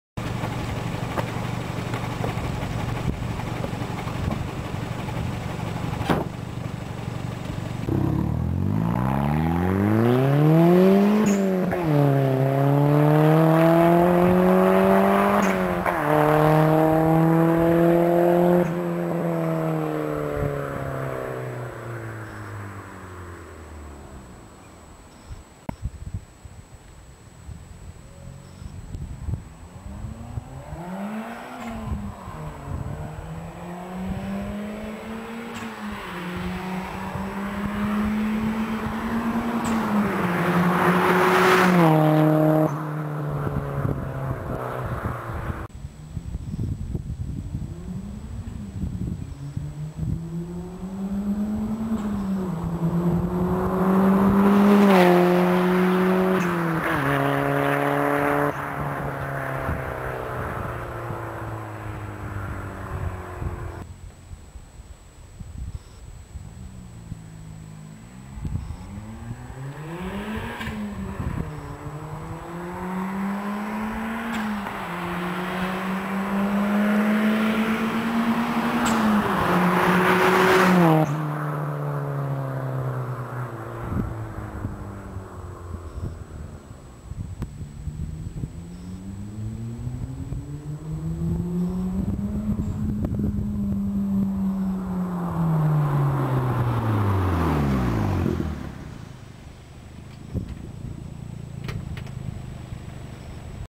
Fly by's Toyota Glanza
Mods:

HKS air filter.
HKS Blow off valve.
HKS Actuator.
WEPR uitlaatspruitstuk.
Roadrunner downpipe.
Turbo timer.
Tanabe Uitlaat.
Varis sideskirts.
Works VSX 15 Inch Gouden velgen.

160 pk.